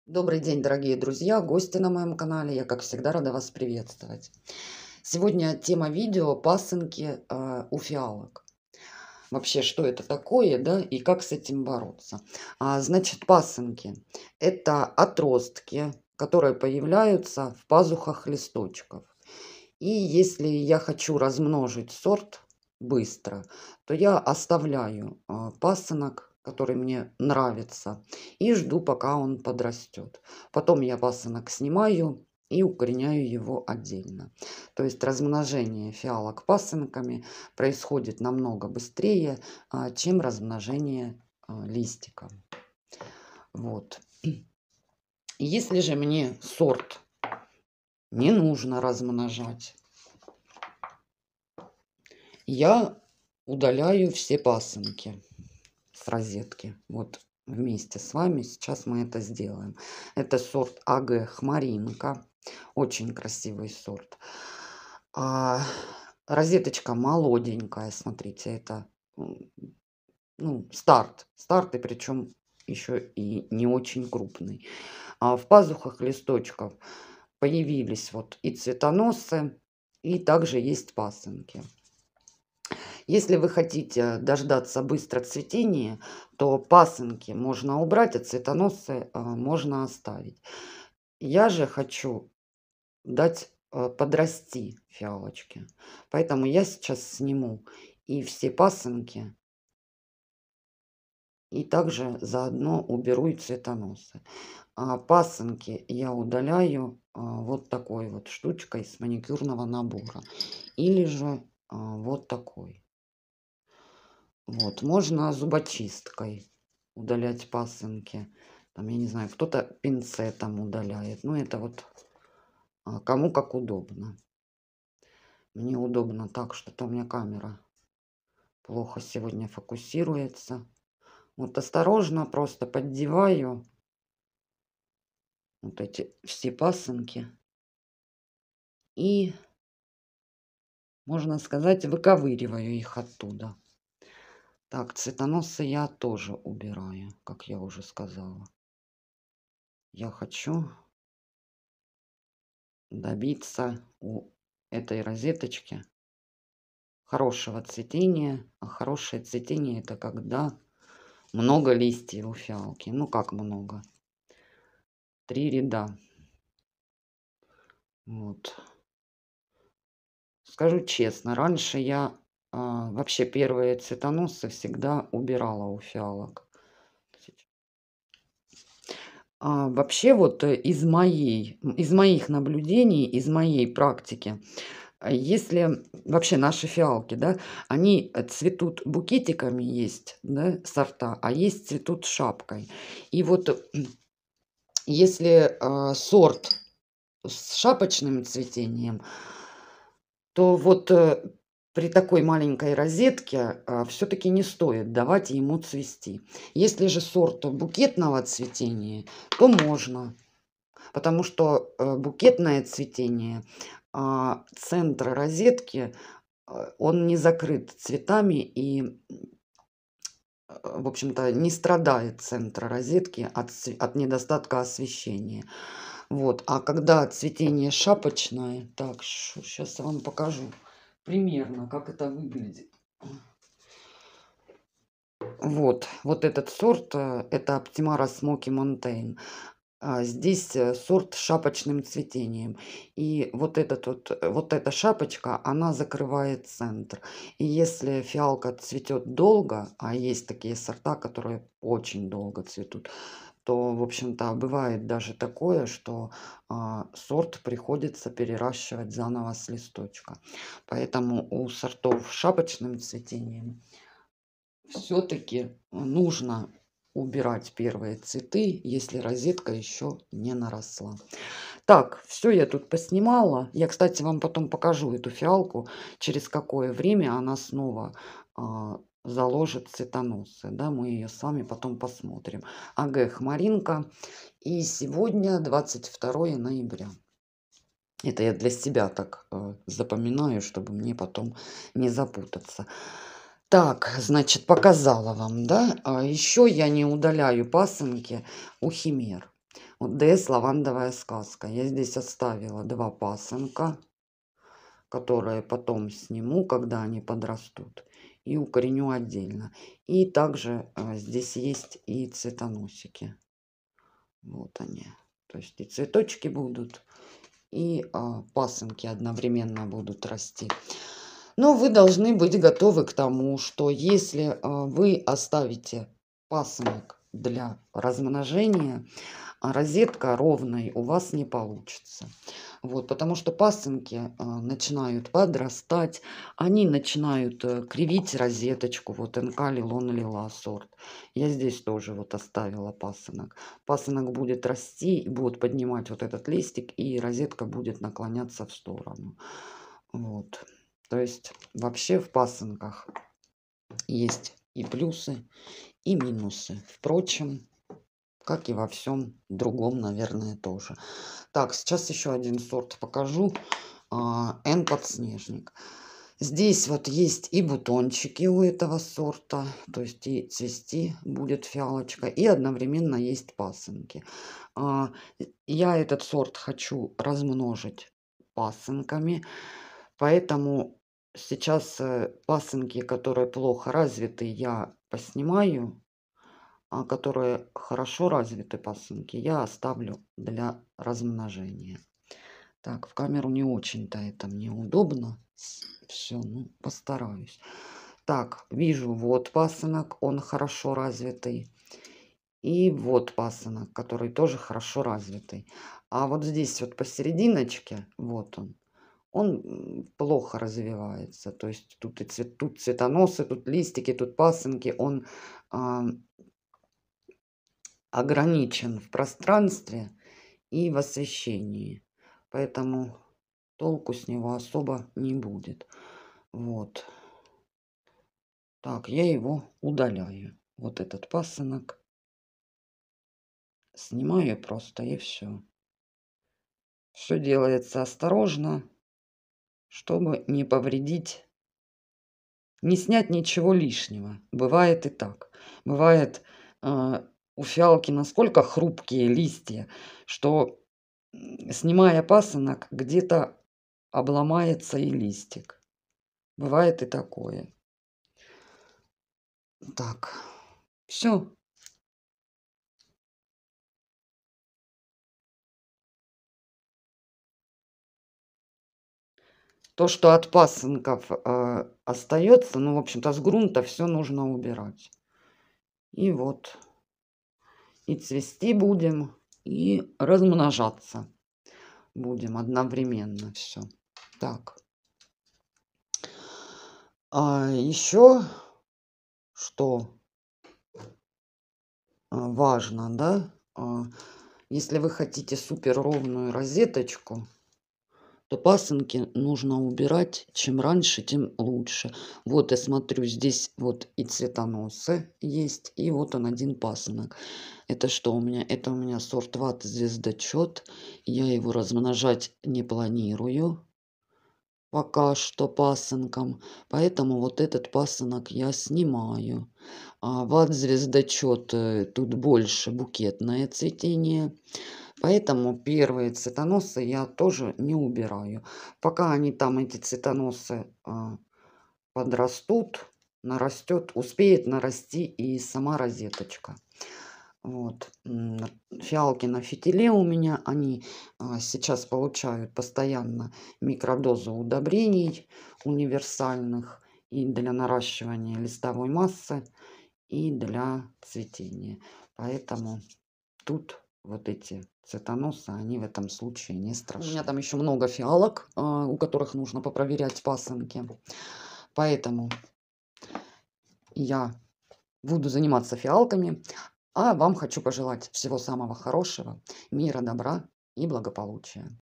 Пасынки у фиалок. Что делать?
Удалять или нет пасынки у фиалок? Как удалять : сразу или постепенно?
Обо всём этом Вы узнаете посмотрев это видео!!!